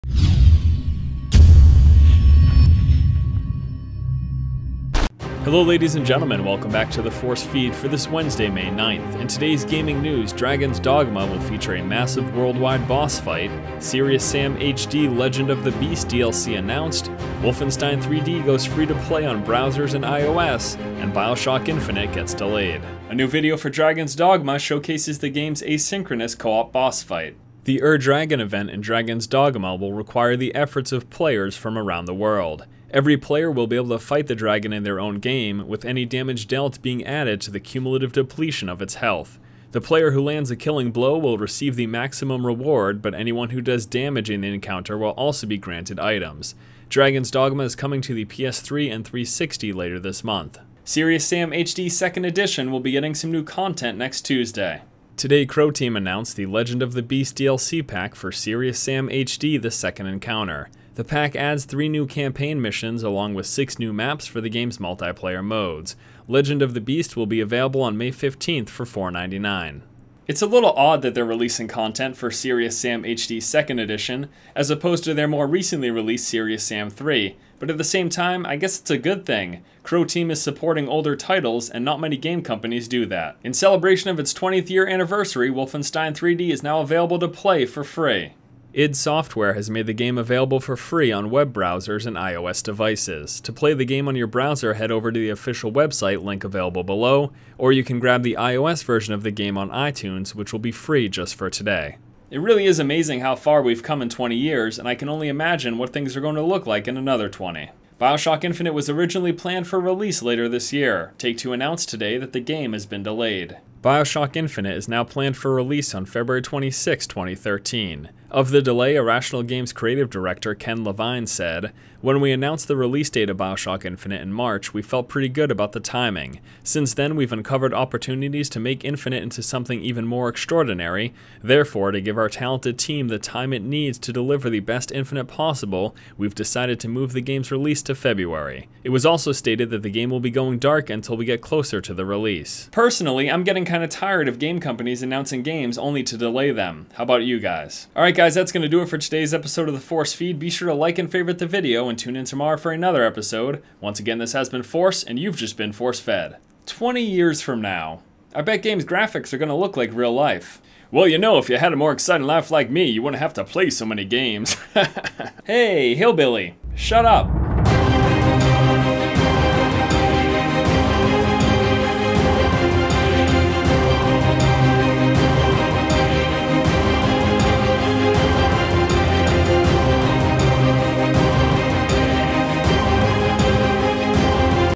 The Force Feed - BioShock Infinite Delay Makes Elizabeth Cry (May 9th 2012)
FSG Presents: The Force Feed

Gaming news and updates Monday - Friday.

Your premier location for strategies and tutorials to the games you love!